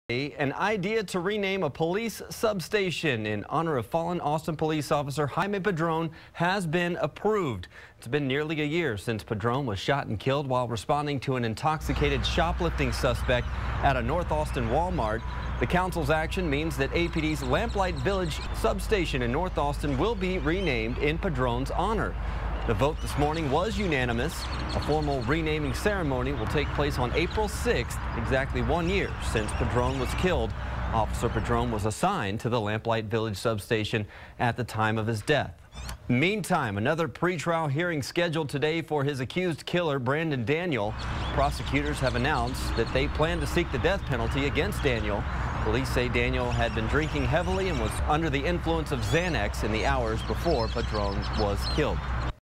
North Austin police substation is named for Padron
The City Council voted Thursday to rename the police substation in North Austin for Jaime Padron, the senior officer who was killed in the line of duty nearly a year ago while responding to a disturbance call at department store.